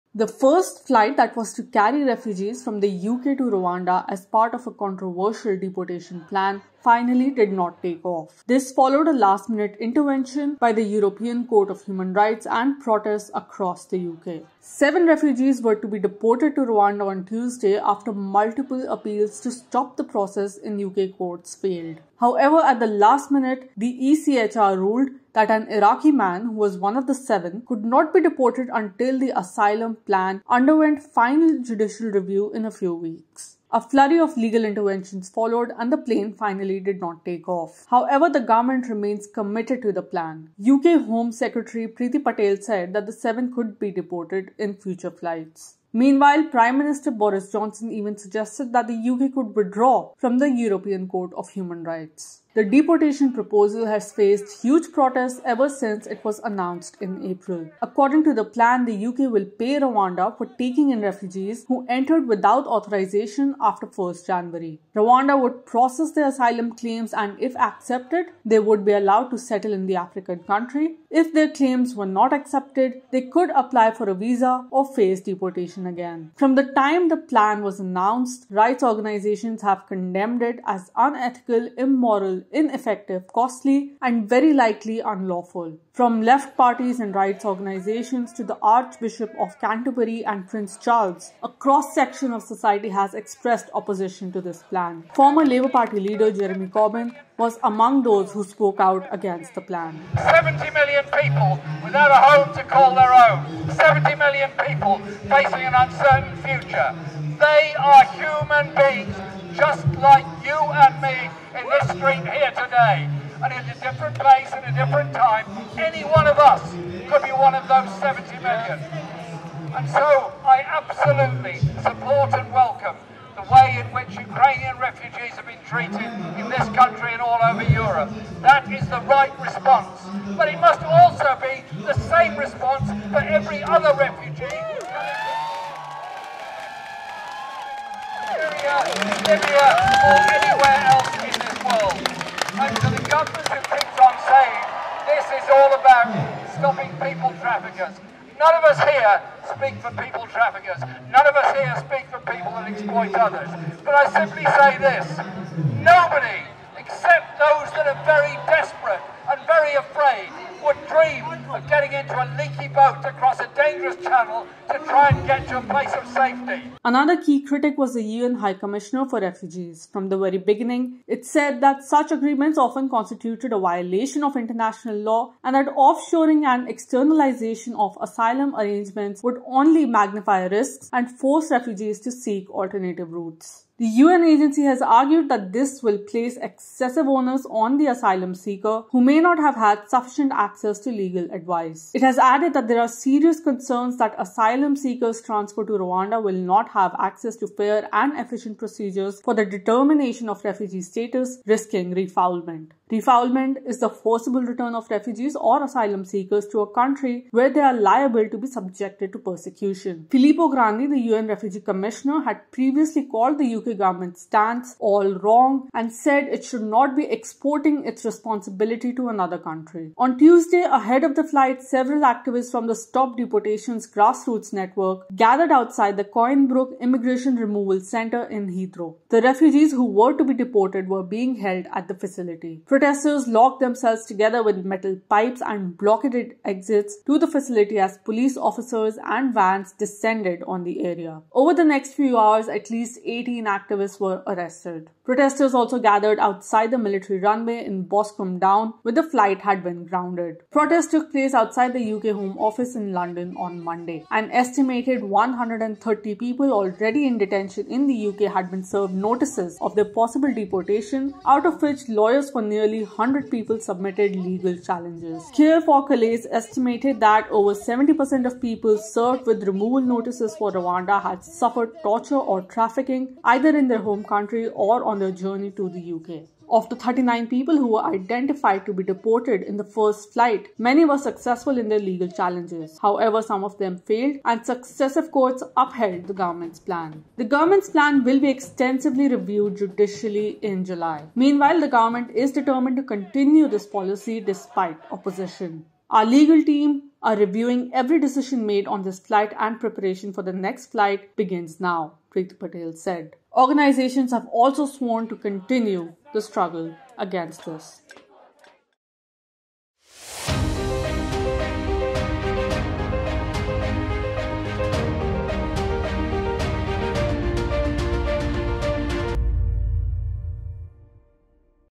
British government's controversial refugee plan faces fierce resistance
The first flight that was to carry refugees from the UK to Rwanda as part of a controversial deportation plan finally did not take off. This followed a last-minute intervention by the European Court of Human Rights (ECHR) and protests across the UK. Seven refugees were to be deported to Rwanda on Tuesday after multiple appeals to stop the process in UK courts failed.
The deportation proposal has faced huge protests ever since it was announced in April. According to the plan, the UK will pay Rwanda for taking in refugees who entered without authorization after January 1.
The government’s plan will be extensively reviewed judicially in July.  Meanwhile, the government is determined to continue this policy despite opposition.